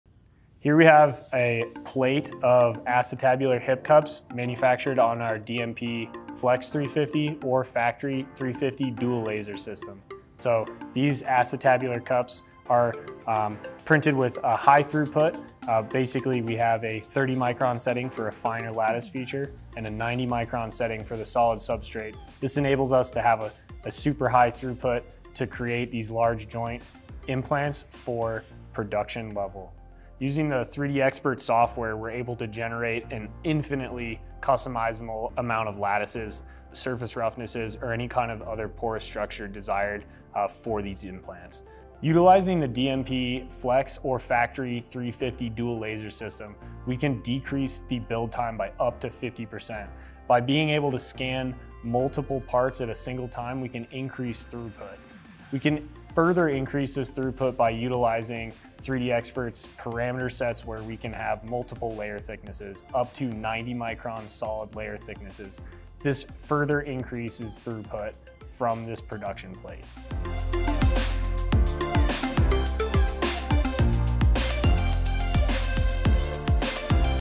3D Printed Acetabular Cup | DMP Metal 3D Printing
Now you can 3D print titanium acetabular hip cups. Produce these large joint applications with intricate lattice structures to support bone-ingrowth twice as fast with our two-laser DMP Flex 350 DUAL platform. 3D System’s Michael Mann explains how the implant’s design and the printer’s advanced capabilities help to make additive manufacturing the ideal choice for hip cups and other medical devices.

Watch the video and be sure to download out new application brief to learn more.